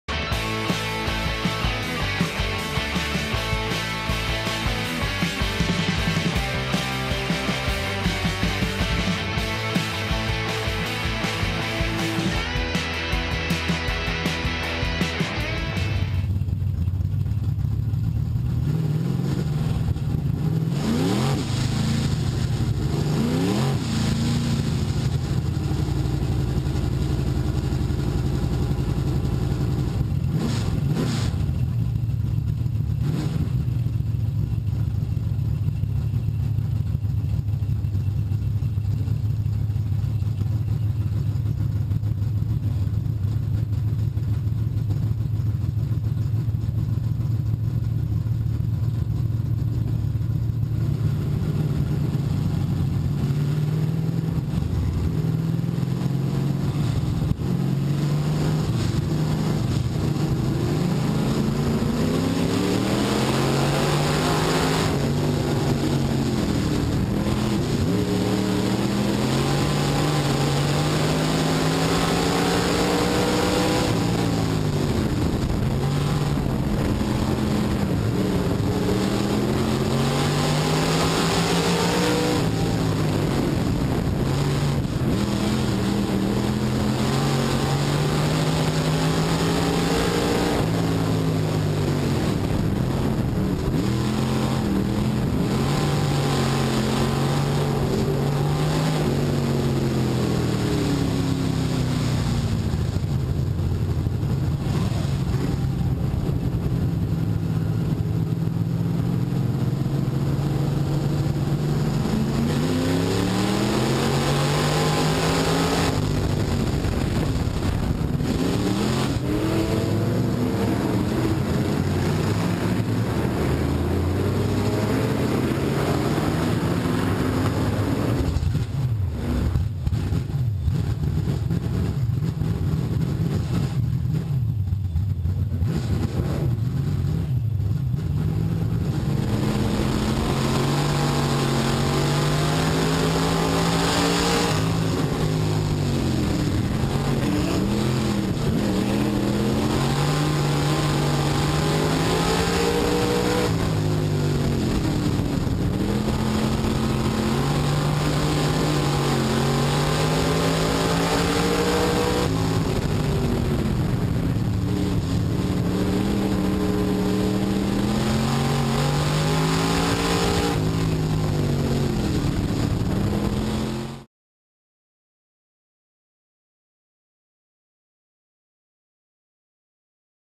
Brad Davis In Car Camera 2nd Feature 9 13 14
Street Stock Driver Brad Davis #88 In Car Camera Action , Street Stock Regular Points race on Sept. 13 , 2014.